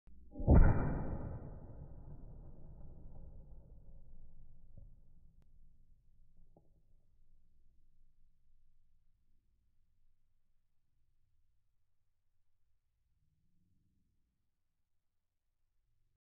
6.5 Creedmoor vapor trial
Vapor trail capture by a Athlon optics spotting scope. 6.5 creedmoor 140eld-m. 600 yards